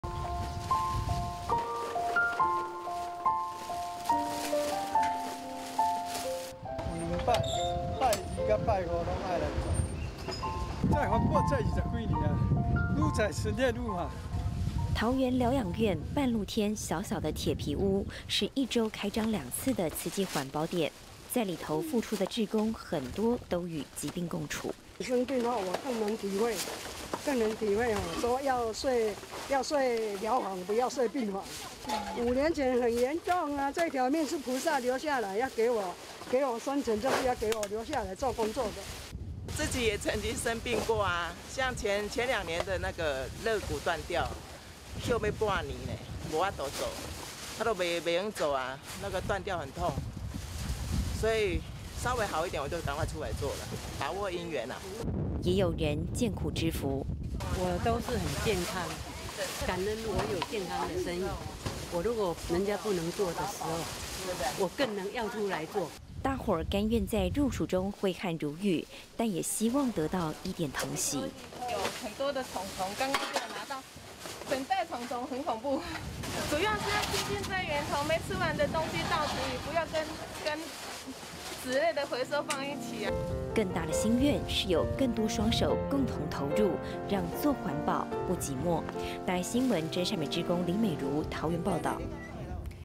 老菩薩把握時間做環保
而這個慈濟環保點，地點很特別，就在桃園療養院。每周兩次做回收，由慈濟志工帶動，院友們，很多身懷病痛，而且很多老菩薩其實身懷病痛，有與癌症共處的，也有大病初癒的，志工說，因為生病過，更能體會時間的可貴，分秒都不能錯過。